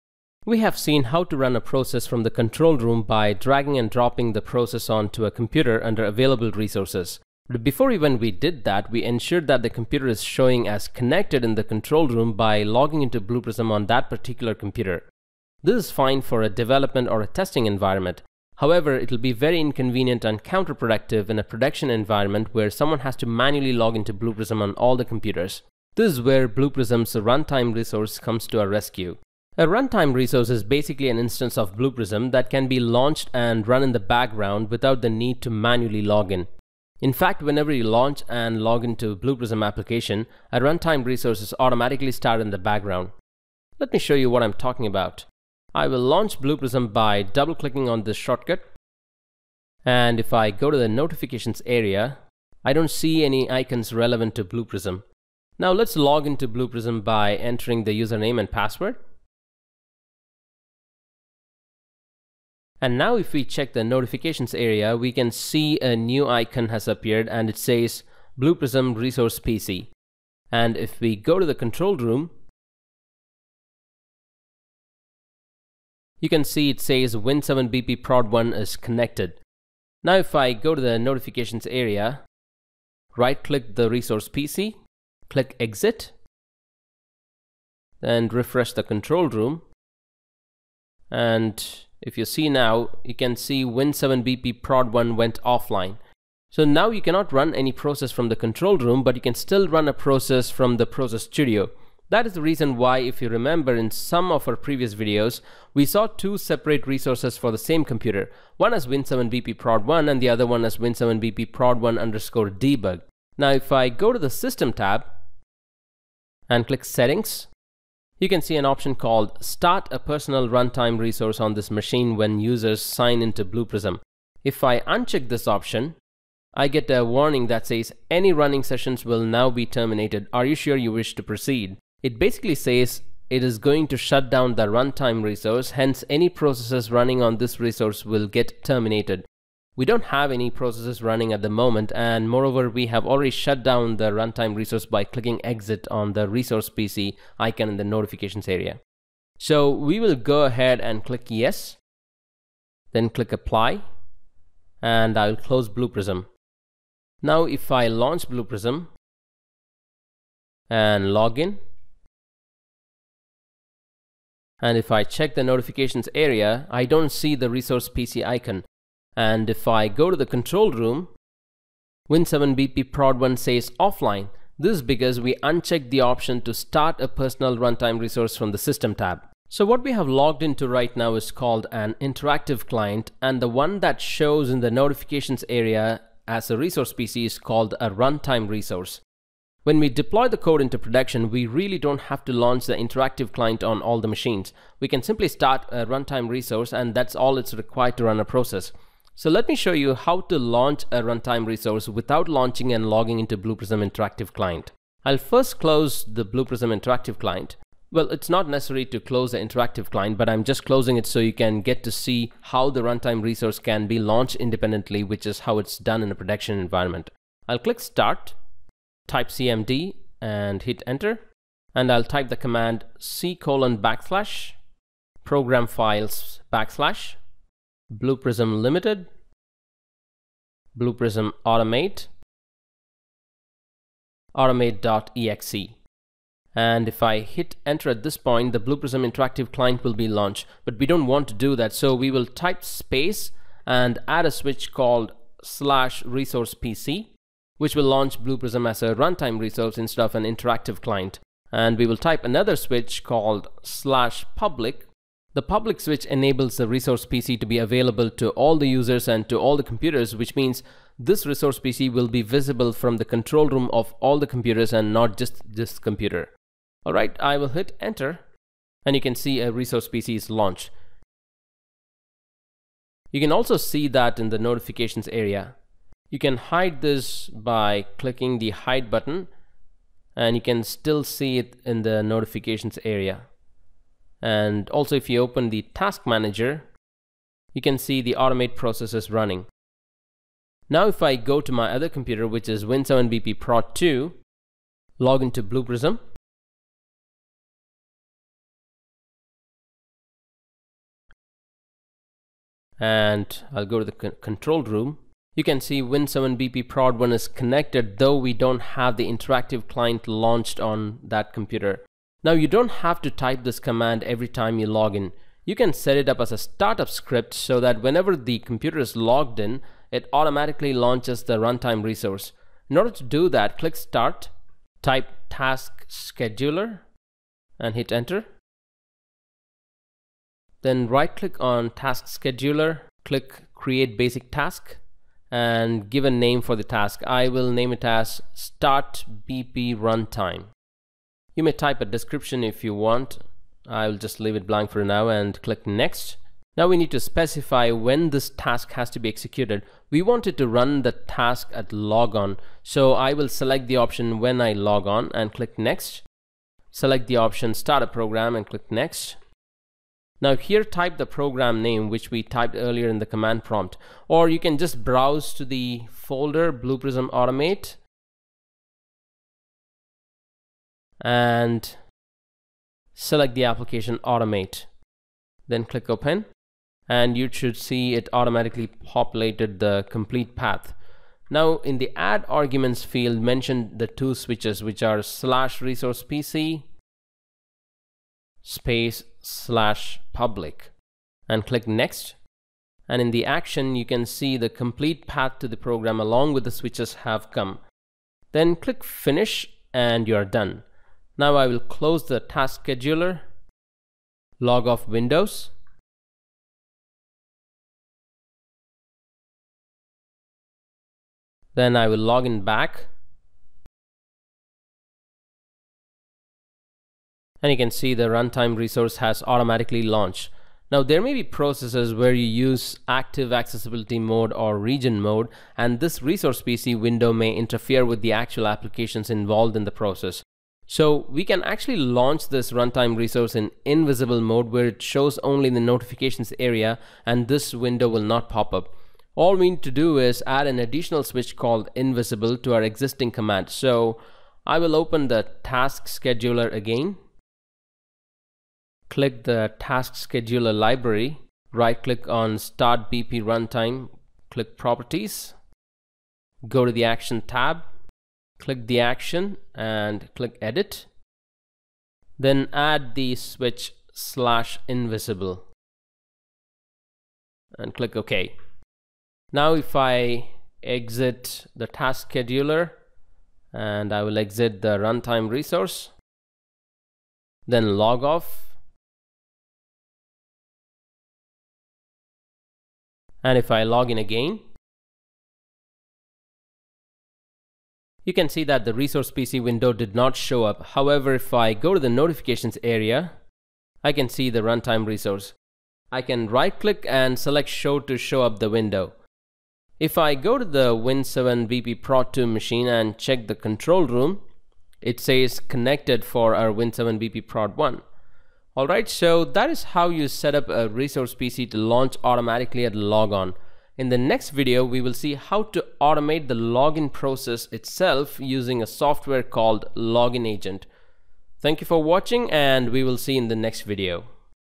Blue Prism Video Tutorial | 042 | Setting up Blue Prism run time resource
This video will give you an indepth understanding of Blue Prism's run time resource and also a complete walk through on how to setup Blue Prism run time resource to launch automatically at login.